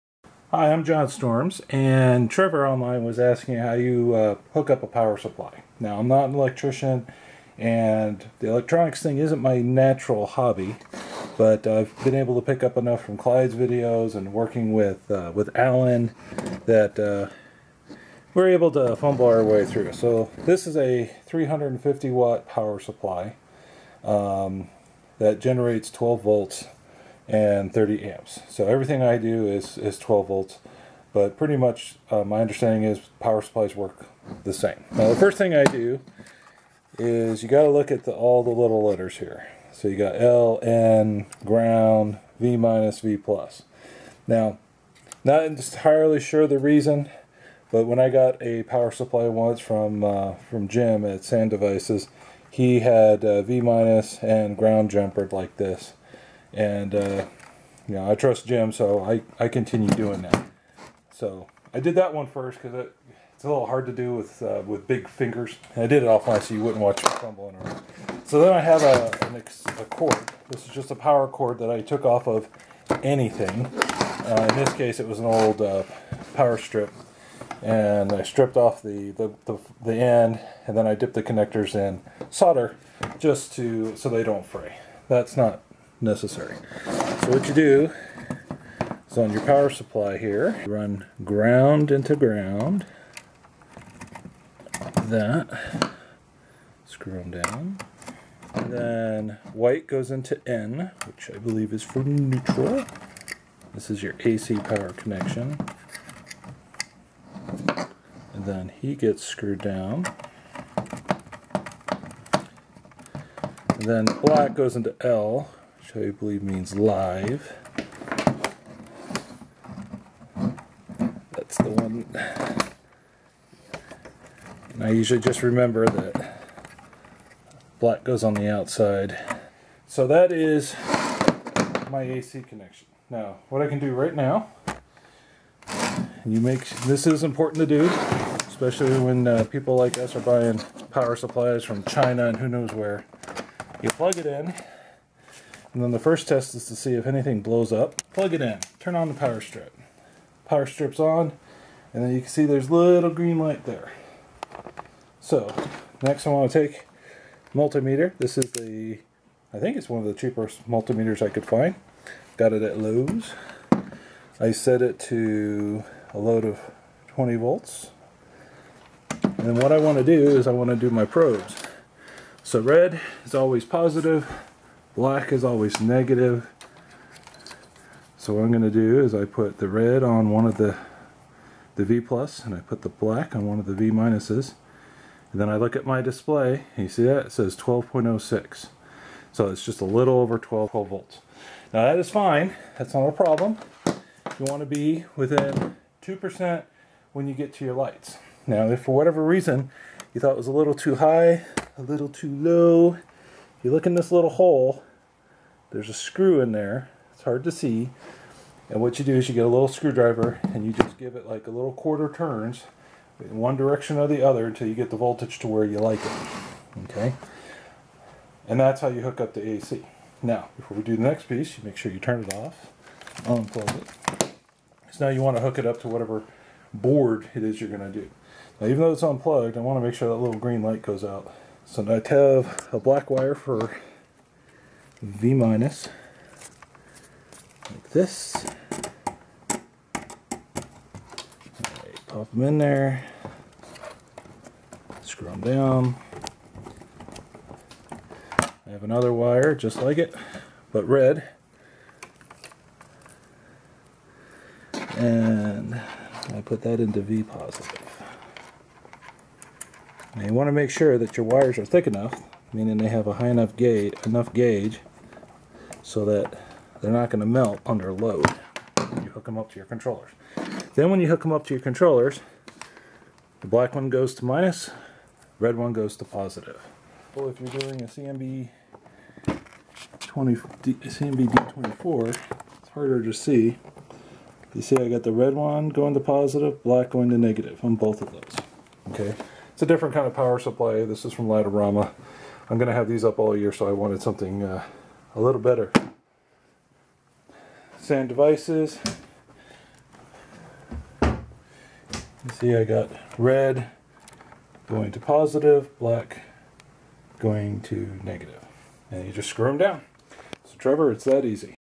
Wiring up a Power Supply (PSU) Don't wire V- to GND! See new video in description.
Do not wire the AC ground to the V- as shown in the video. I bought this PSU along with a San Devices controller and it came that way.

12v 350 watt power supplies are very common when using various light controller boards (SanDevices, Falcon16, LOR CMB24D, etc.). This video does a quick walk through of hooking up and testing a PSU to AC input and DC output.

I buy my PSU's from Ray Wu or from Seasonal Entertainment.
I bought my Southwire Digital Multimeter from Lowes.